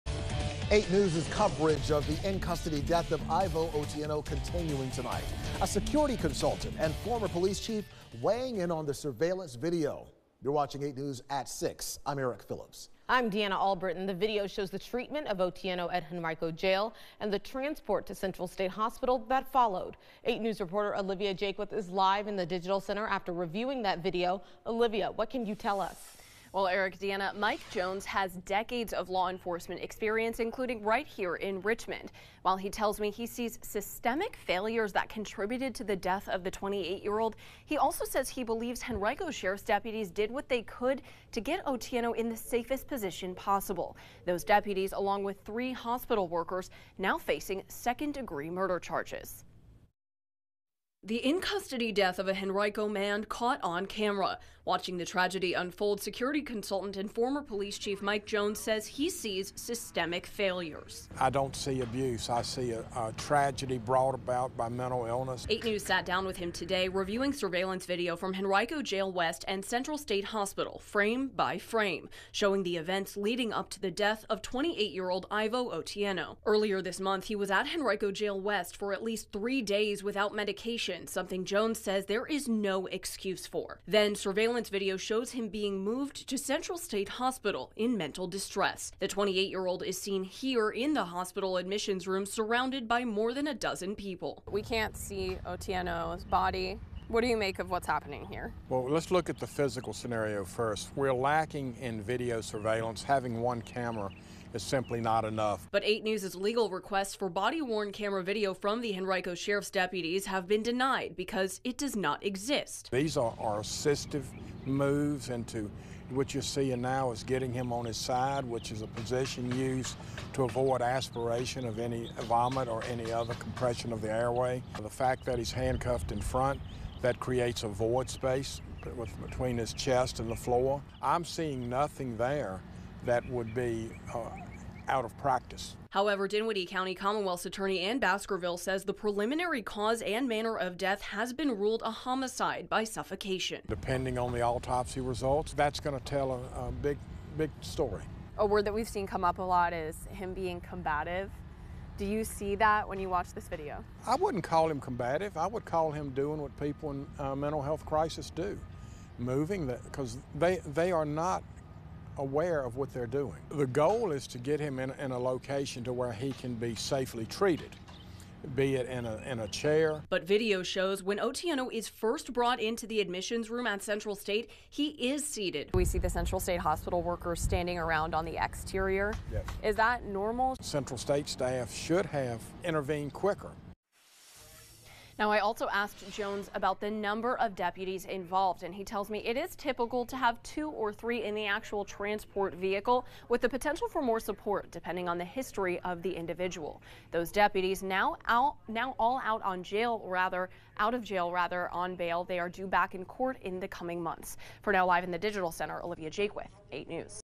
Former Virginia police chief breaks down surveillance video in death of Irvo Otieno
Following the in-custody death of Irvo Otieno, 28, at Central State Hospital earlier this month, surveillance video of his transfer from Henrico Jail west to the hospital has been released and has garnered national attention.

8News spoke with Virginia security consultant and former police chief Mike Jones on Friday, March 24, and reviewed the surveillance video that shows Otieno in Henrico Jail West and Central State Hospital.

Before Friday, Jones said he had watched the video several times over.

“I don’t see abuse. I see a tragedy brought about by mental illness, which is endemic in our society,” Jones said. “Central State staff should have intervened quicker."